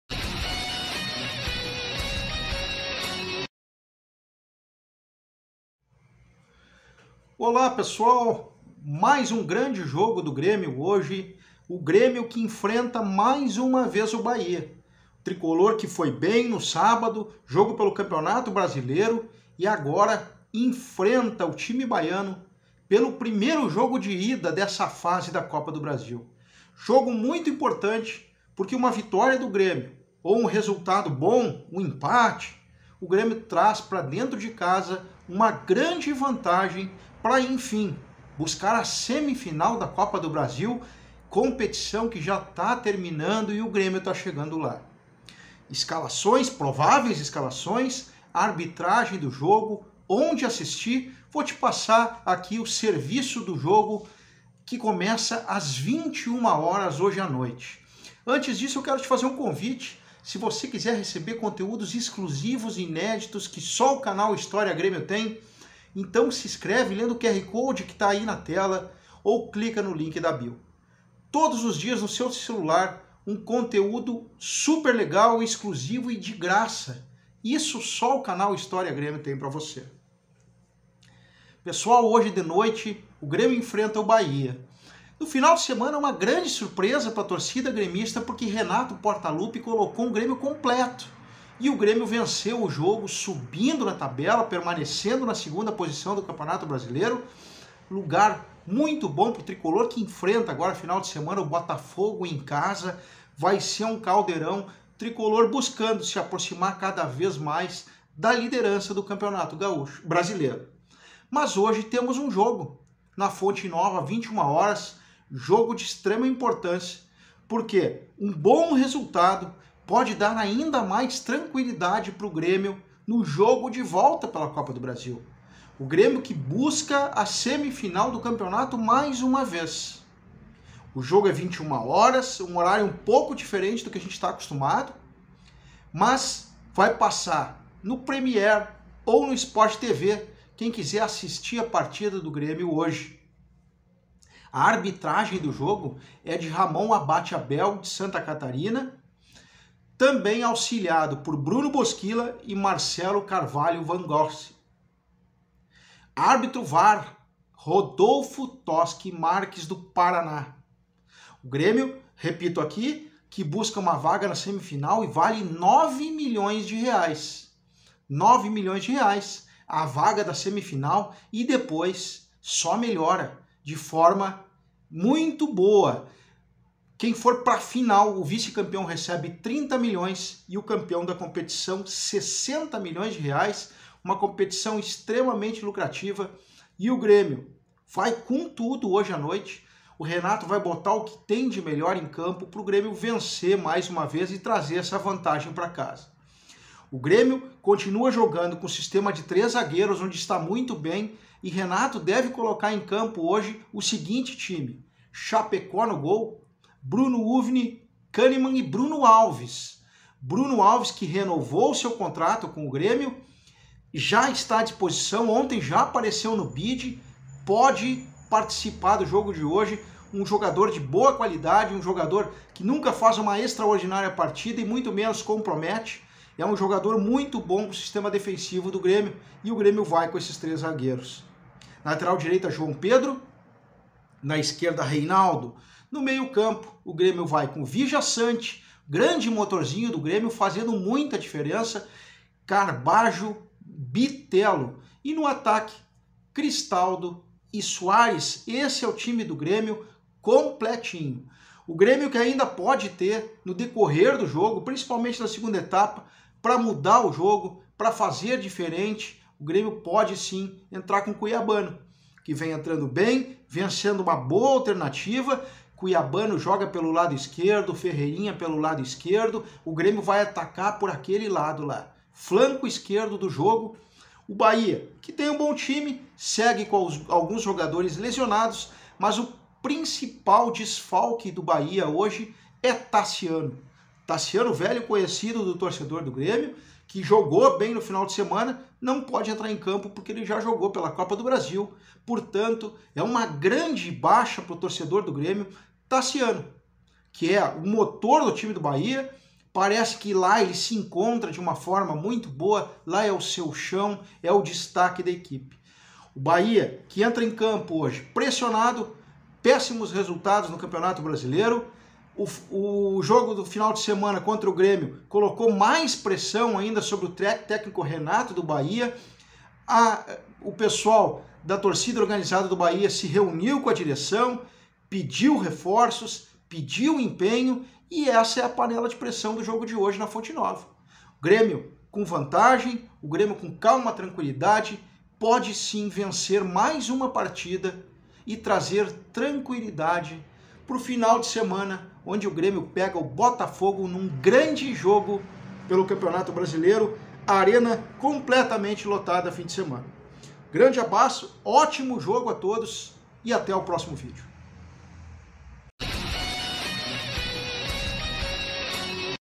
BAHIA X GRÊMIO: ONDE ASSISTIR, ARBITRAGEM, ESCALAÇÕES
Grande jogo logo mais entre Bahia x Grêmio.

Educacoin, a primeira criptomoeda brasileira voltada ao ramo da educação.